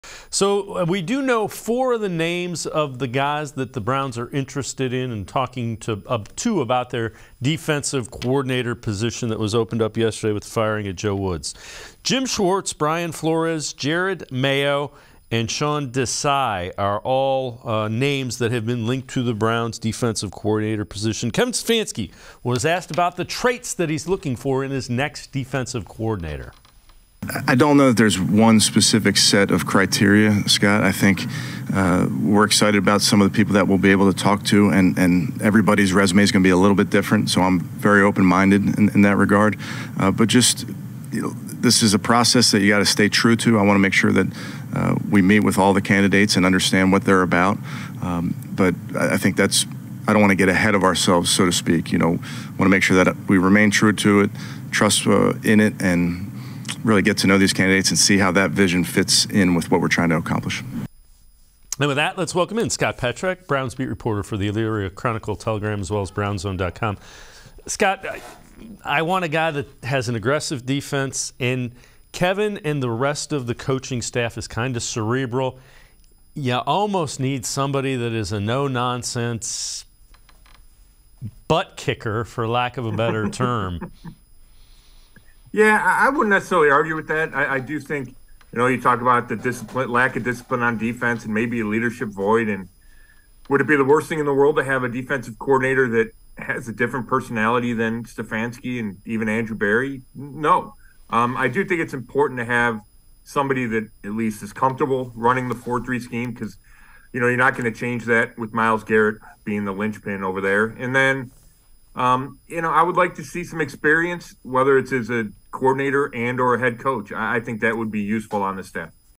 Traits Kevin Stefanski is Looking for in the Browns Next Defensive Coordinator - Sports4CLE, 1/10/23
Dave Bacon and Scott Petrak respond to what Kevin Stefanski had to say on what he is looking for in his next defensive coordinator.